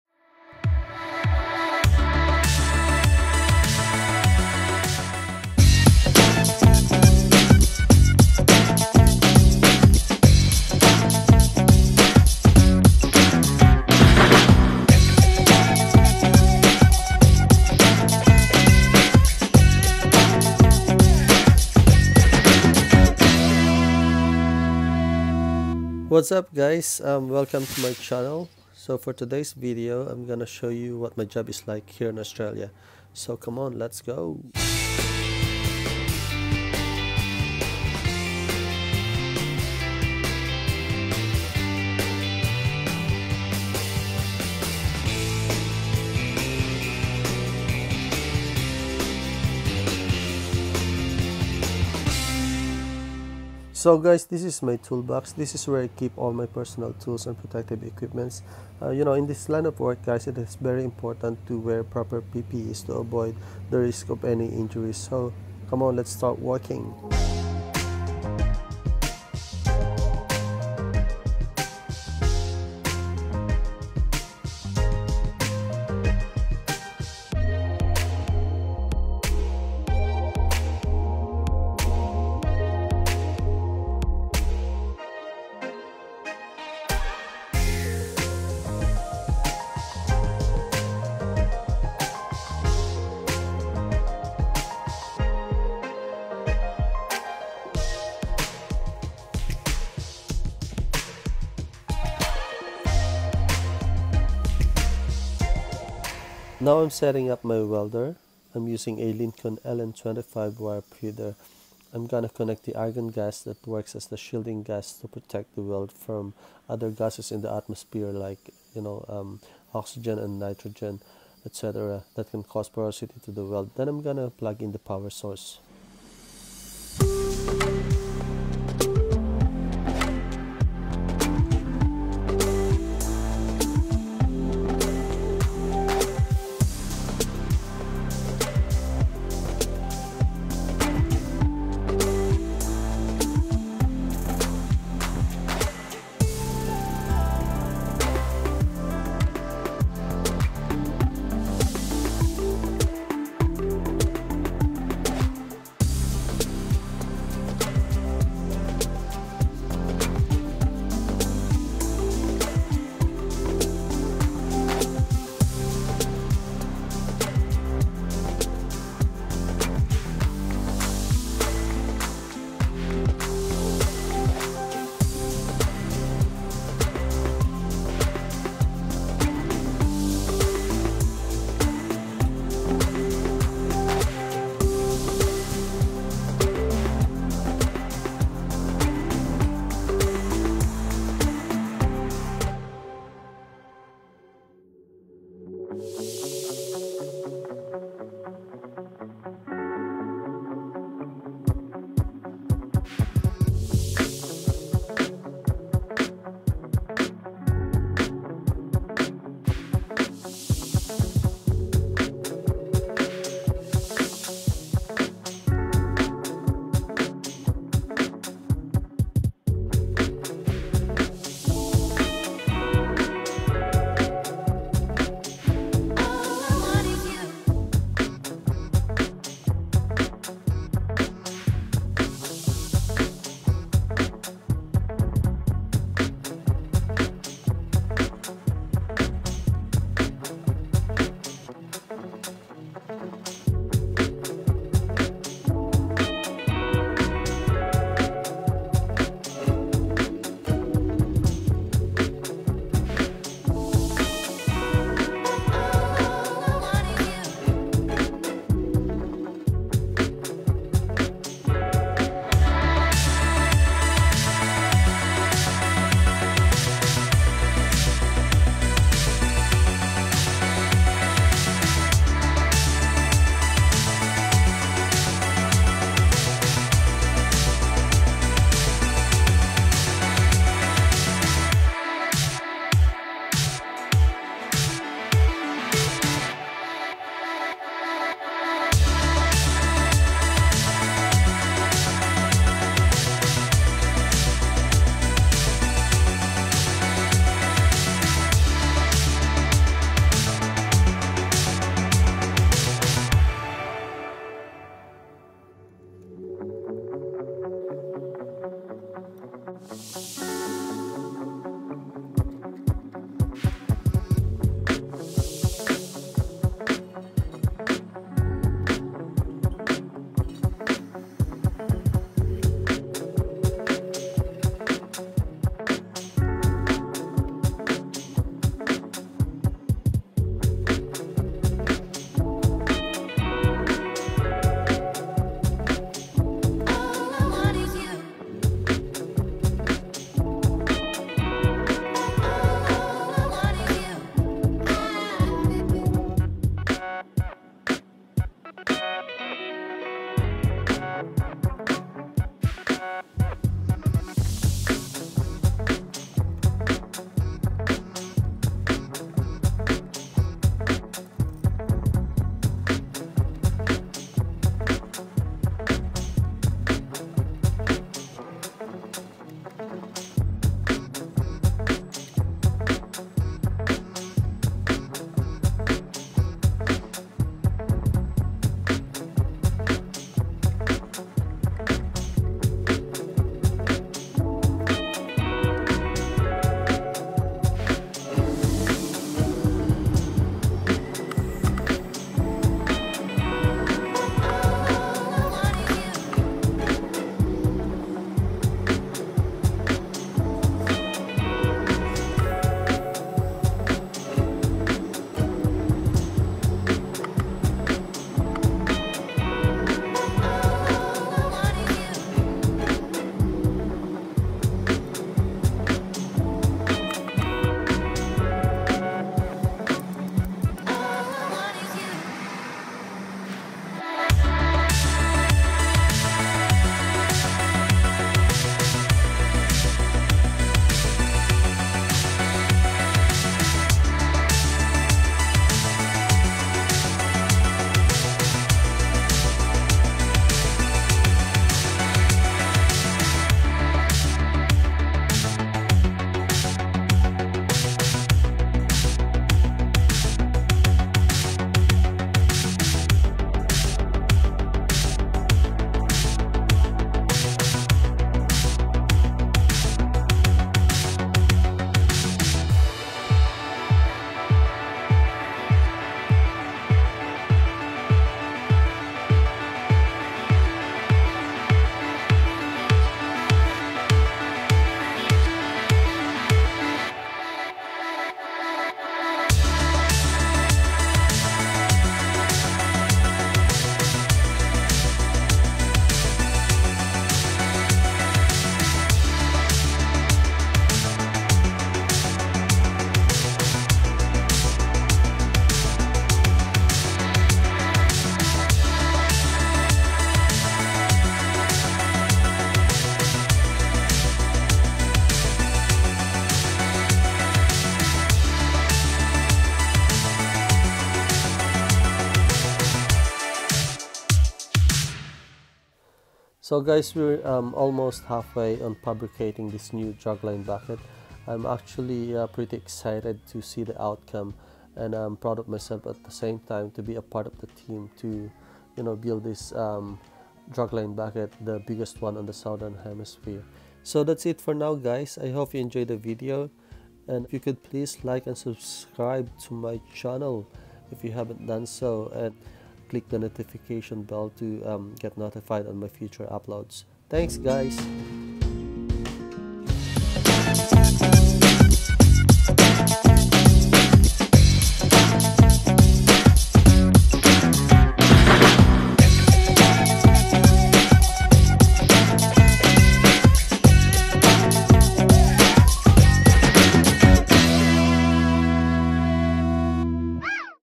WELDING FABRICATION AUSTRALIA
Hey guys on this video I will show you around my work place. We are now fabricating the biggest dragline bucket in the Southern Hemisphere and Im really proud to be a part of the team to build this. We are using flux core arc welding on these job as it offers high welding productivity which are commonly used on heavy fabrications. What do you guys think? If you have any questions or suggestions please leave a comment on the comment section. Thank you!